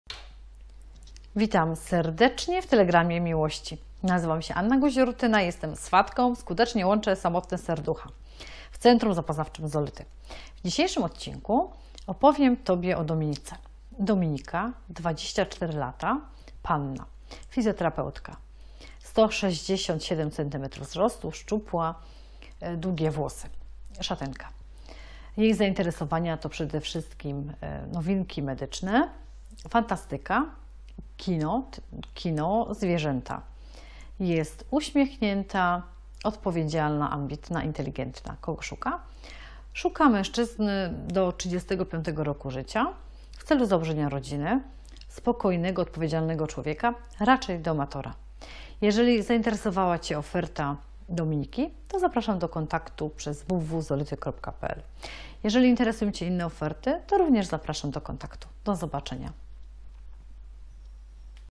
Telegram Miłości 23 Dominika 24 Lata
Zapraszam na dwudziesty trzeci odcinek Telegramu Miłości, w którym przedstawiam Dominikę 24 Lata